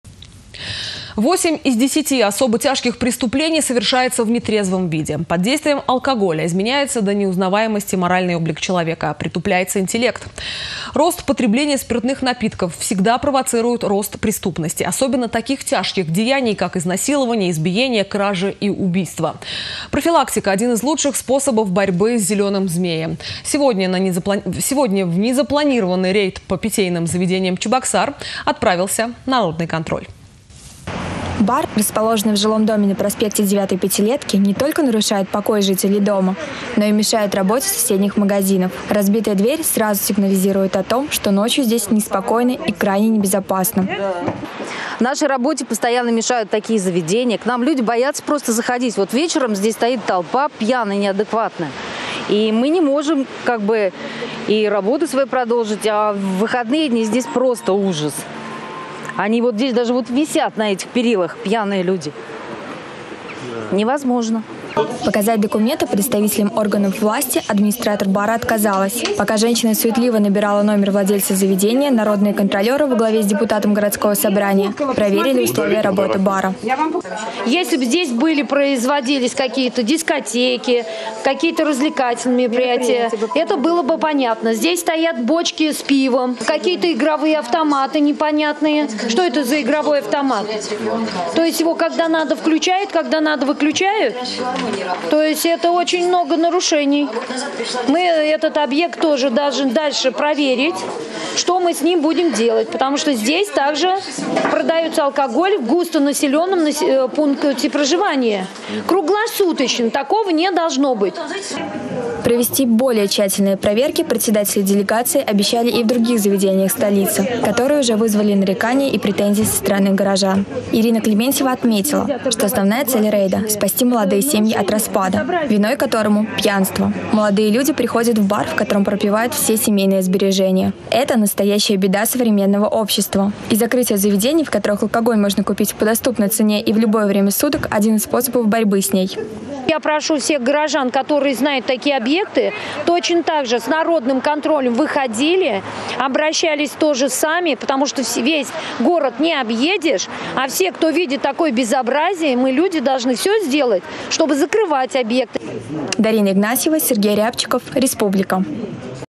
В незапланированный рейд по питейным заведениям столицы отправился народный контроль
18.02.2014
8 из 10 особо тяжких преступлений совершается в нетрезвом виде. Человек под действием алкоголя меняется и внешне, и внутренне. Изменяется до неузнаваемости его моральный облик, притупляется интеллект. Эти изменения и способствуют совершению преступлений. Рост потребления спиртных напитков всегда провоцирует рост преступности, особенно таких тяжких преступлений, как изнасилование, избиения, кражи и убийства. Именно поэтому в незапланированный рейд по питейным заведениям столицы отправился народный контроль.